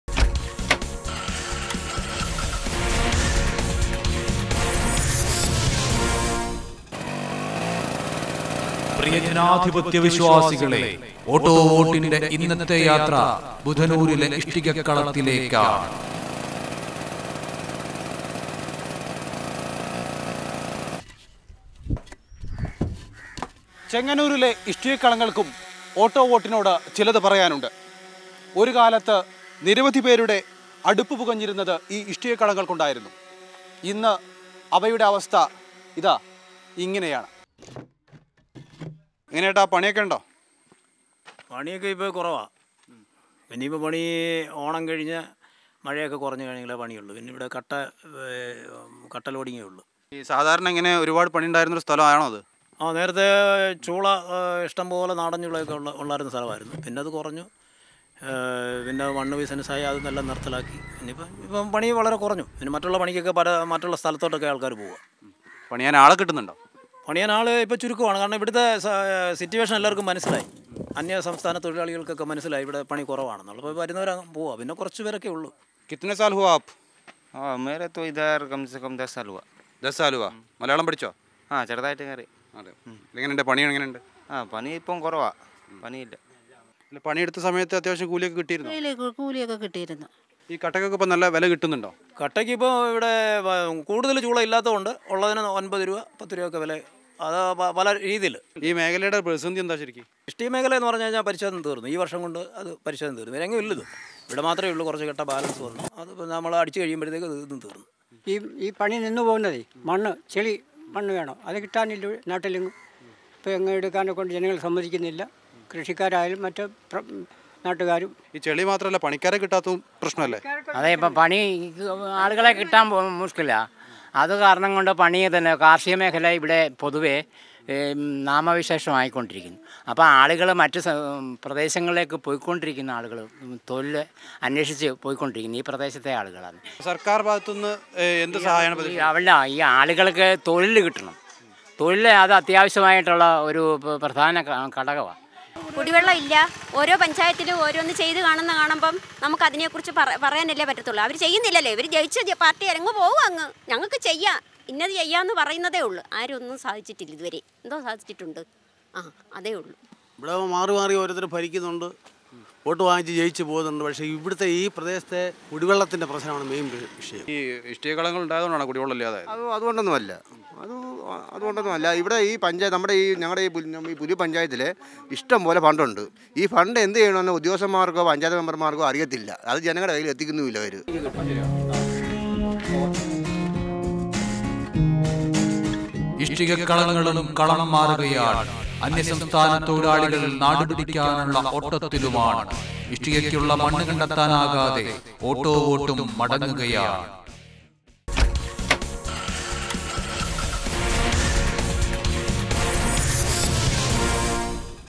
Auto Vote At Brick Kiln In Budhanoor| Mathrubhumi News
Auto Vote shares the problems and demands of the workers from a brick kiln at Budhanoor.